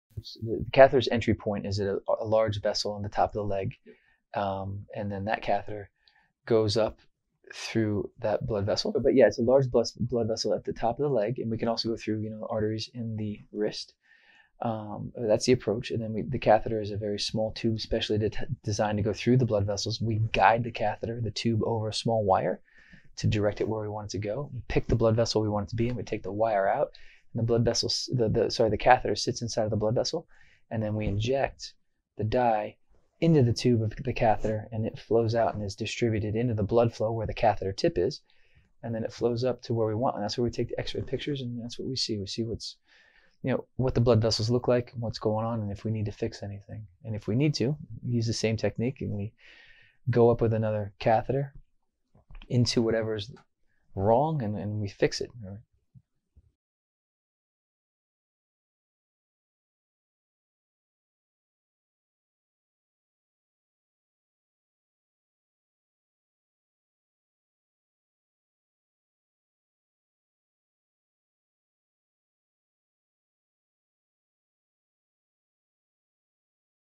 The Dye Used for an Angiogram is Injected through the Catheter
Dr. Kyle Chapple discusses how the dye used for an Angiogram is Injected through the Catheter.

Dr. Kyle Chapple
Atlantic NeuroSurgical Specialists
310 Madison Ave
Suite 300
Morristown, New Jersey 07960